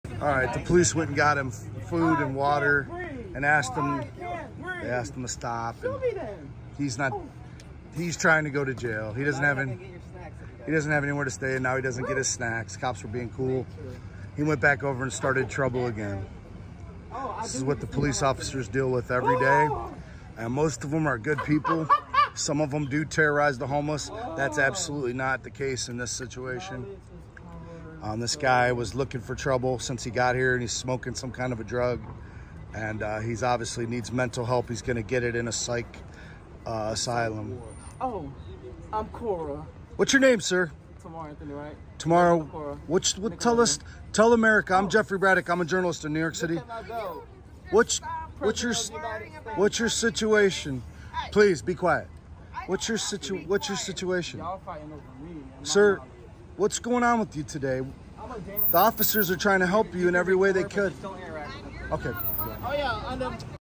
BRADENTON HOMELESS MAN.. FAKES BEING BRUTALIZED BY COPS!! CAUGHT ON CAMERA ‼️
BRADENTON HOMELESS MAN CAUGHT FAKING POLICE BRUTALITY ‼️JEFFREY BRADAC INVESTIGATIVE JOURNALIST IN FLORIDA ‼️